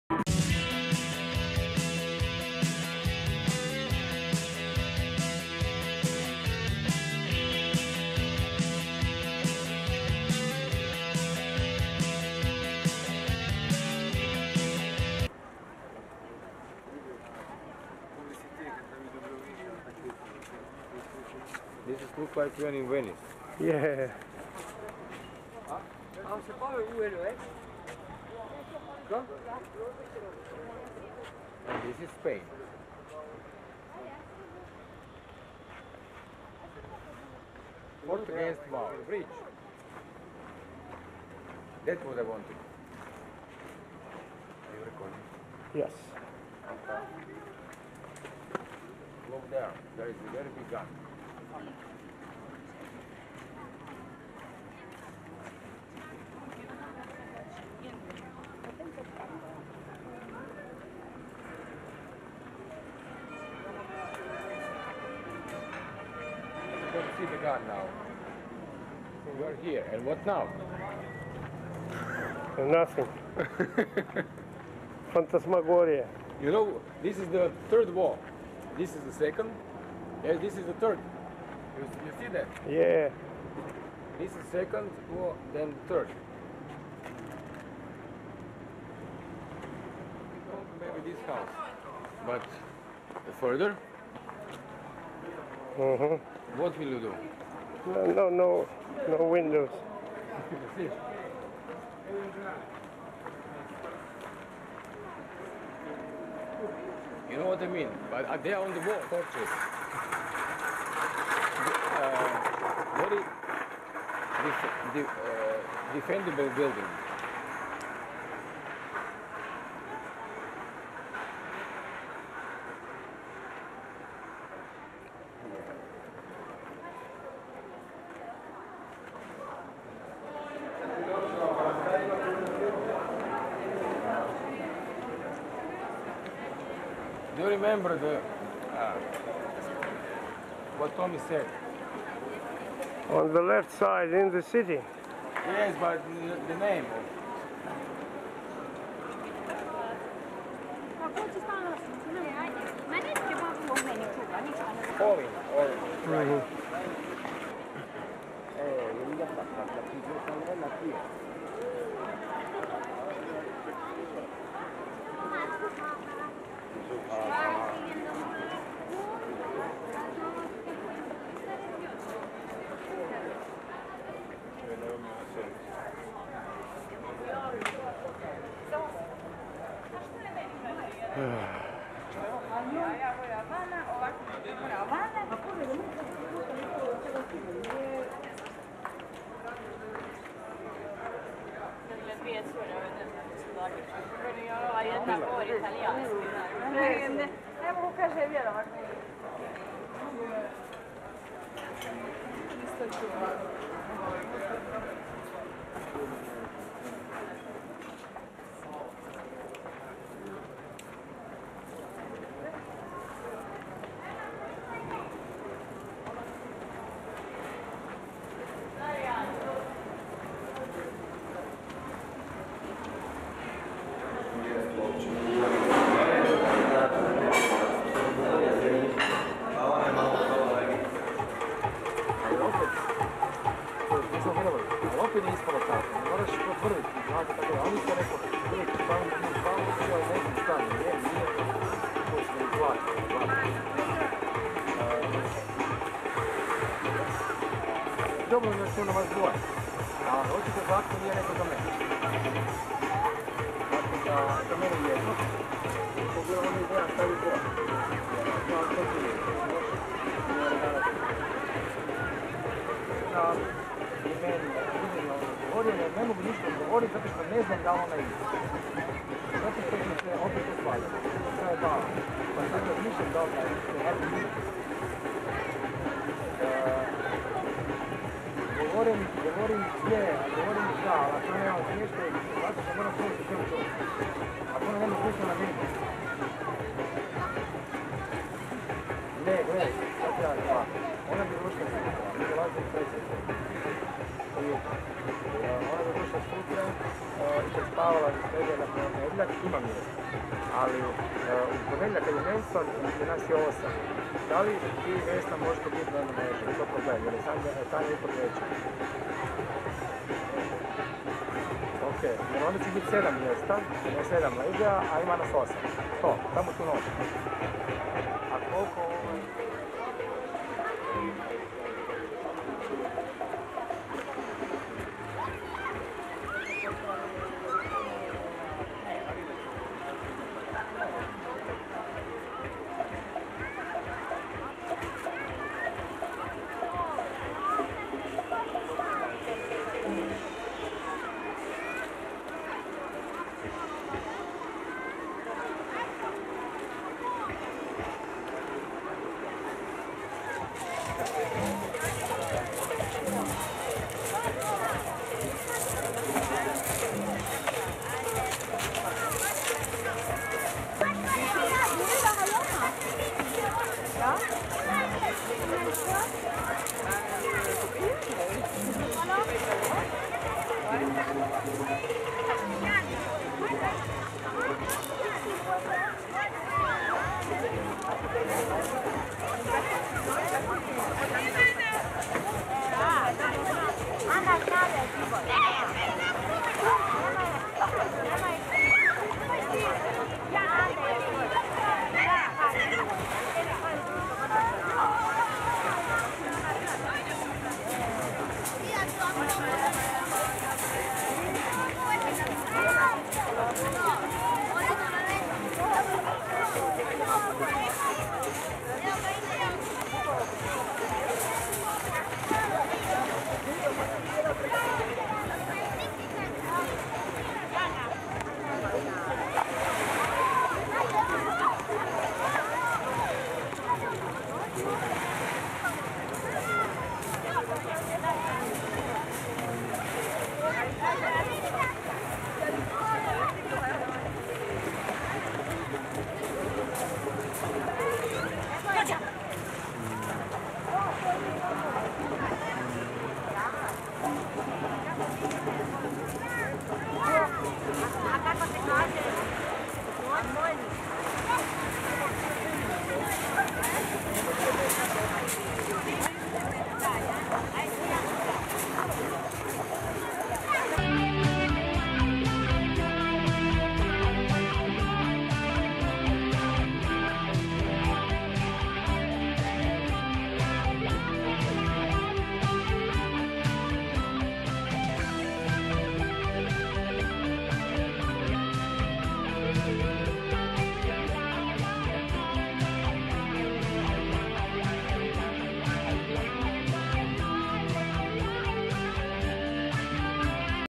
Jadran Tour 2005 - The Old City & beach
The morning after  performance.20.08.2005,  Dubrovnik, Croatia. Audio - Phantasmagoria.